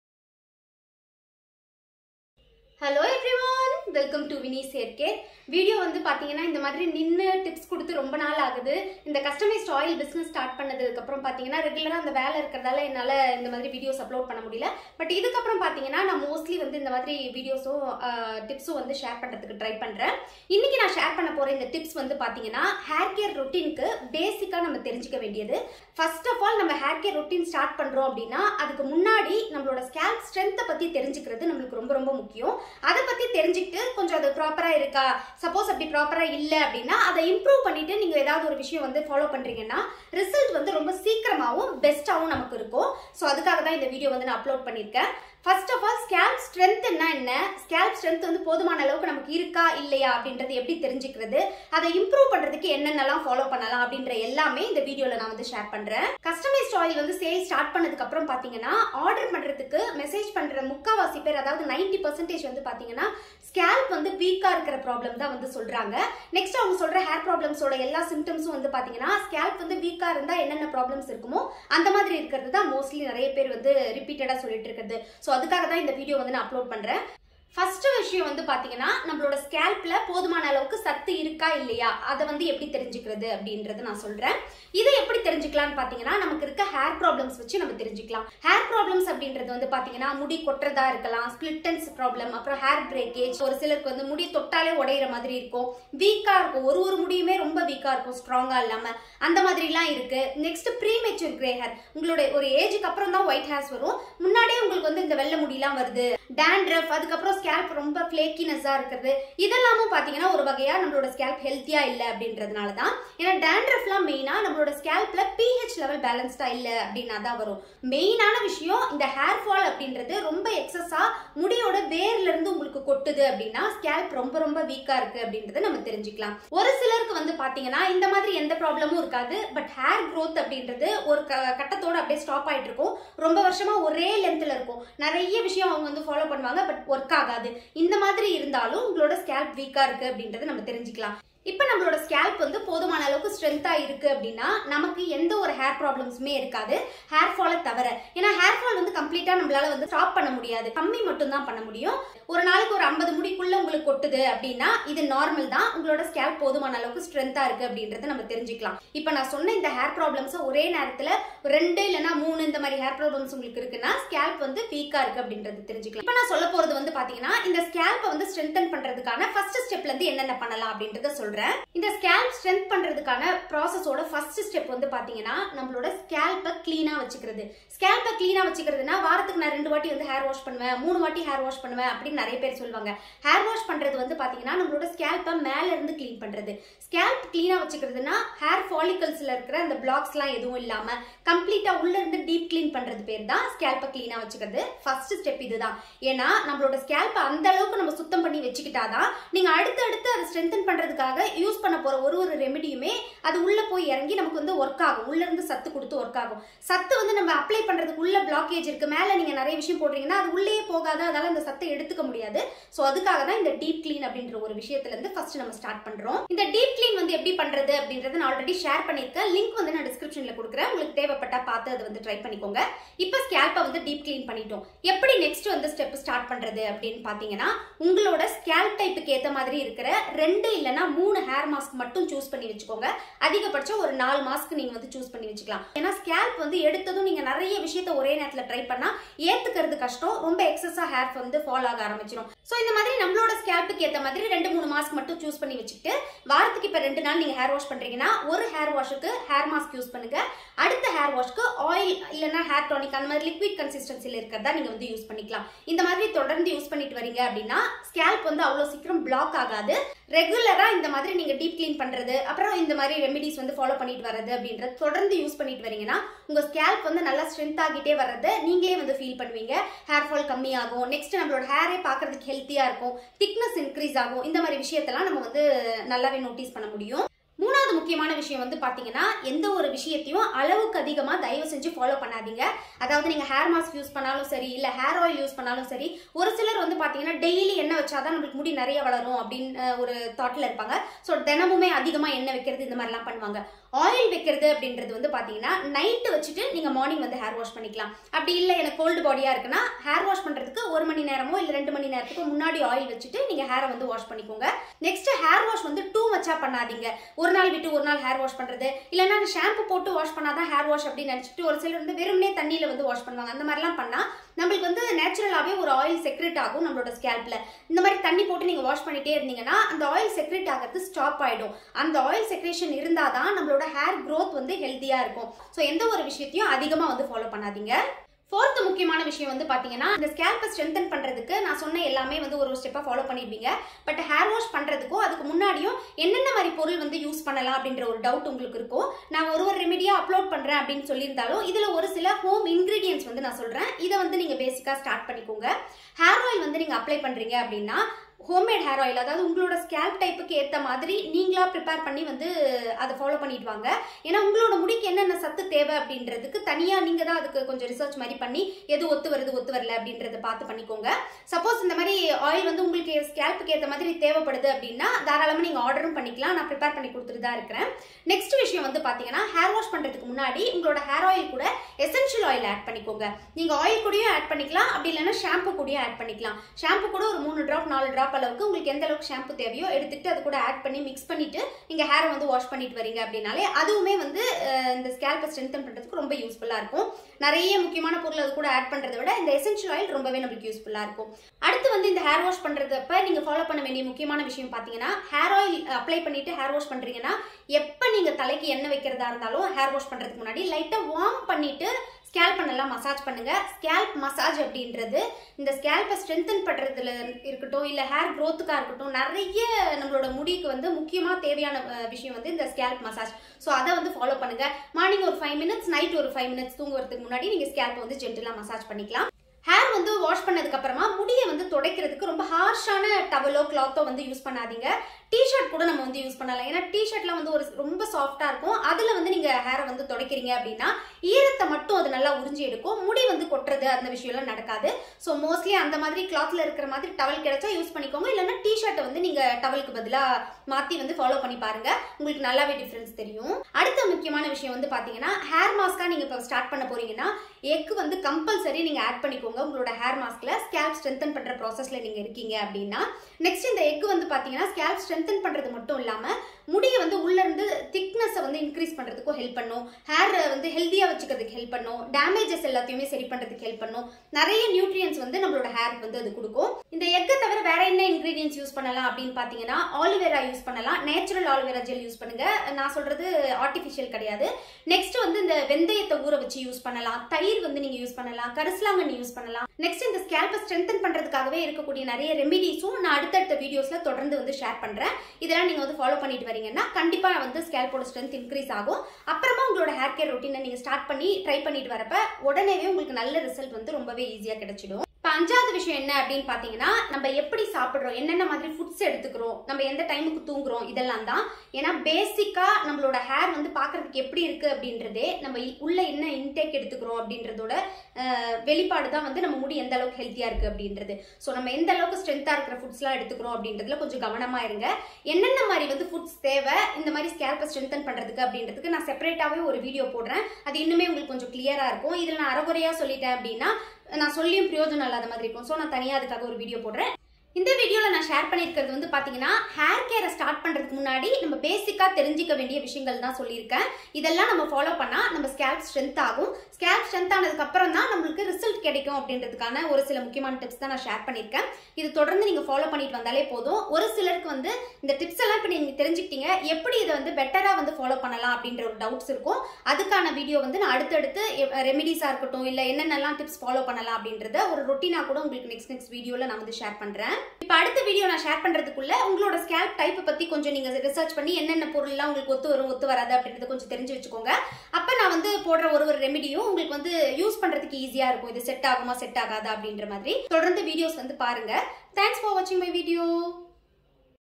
All About Scalp Strength | How to Know Your Scalp Strength & How to Increase Your Scalp Strength
Scalp Strengthening Challenge

To place your customized hair oil order Contact,

Thank you so much for the all the love and support.. It means a lot..